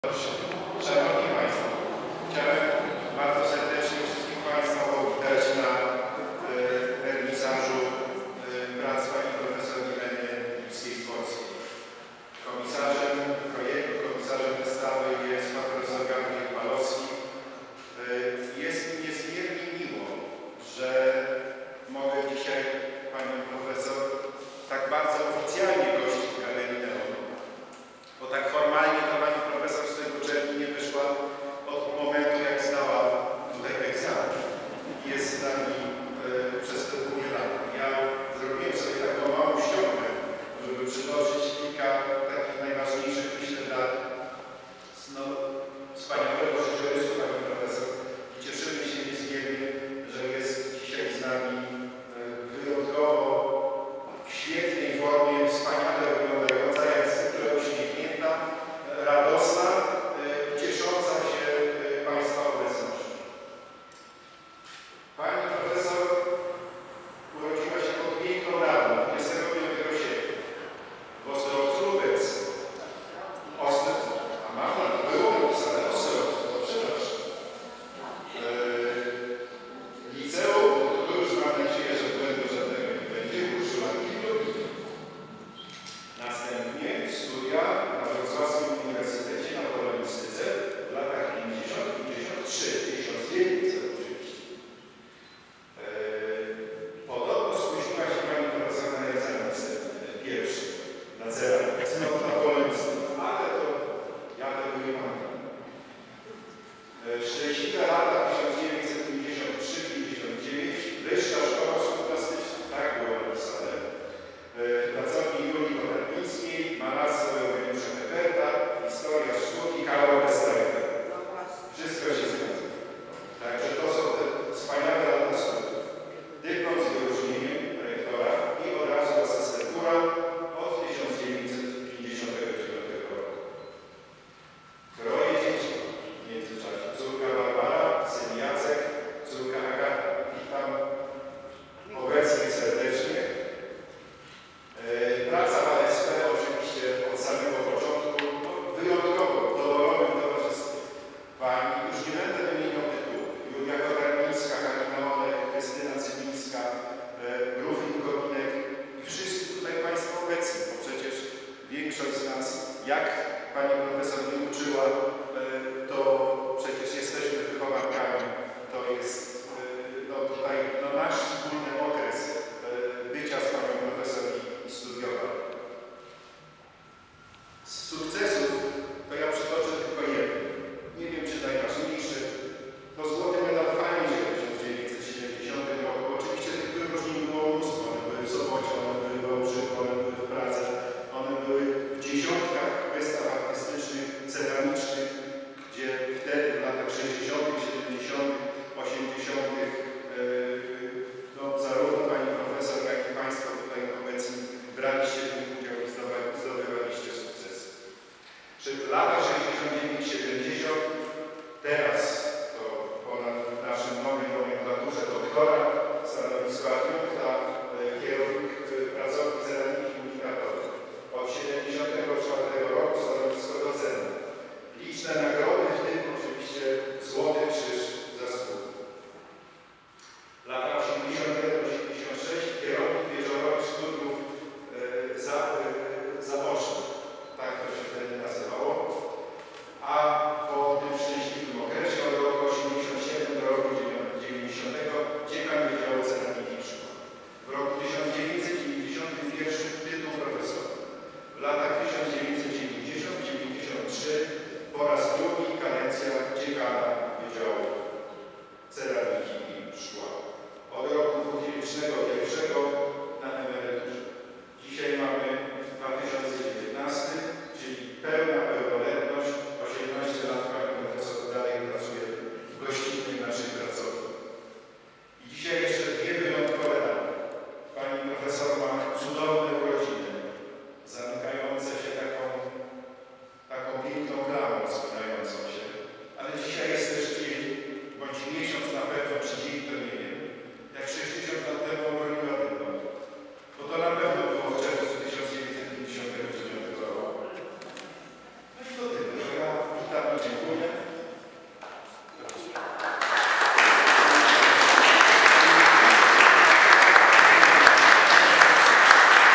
Lipska-Zworska Irena – "Moje światy", 11 czerwca 2019
Laudację w 88-lecie urodzin i 60-lecie uzyskania dyplomu ASP przez prof. Irenę Lipską-Zworską wygłosił prof. Piotr Kielan – Rektor Akademii Sztuk Pięknych im. Eugeniusza Gepperta we Wrocławiu podczas wystawy – "Moje światy" w Galerii ASP Neon.